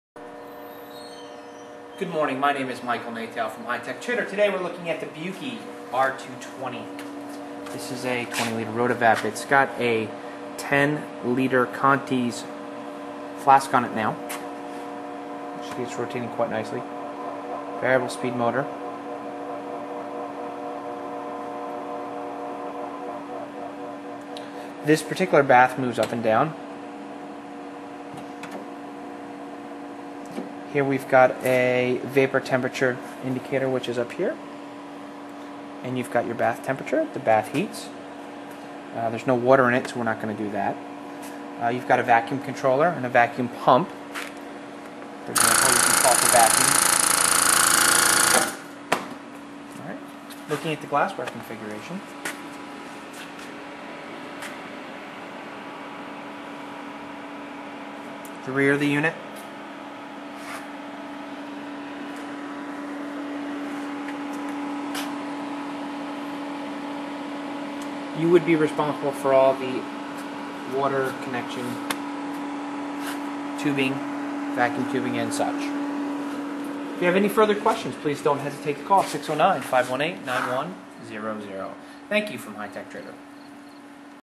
Buchi R-220 Rotovap
Upload to 2010/09/29, Büchi Rotary Evaporator  R-220, Large-Scale, Descending Glass Assembly with 2 Bullfrog Condensers, ( for restricted height application)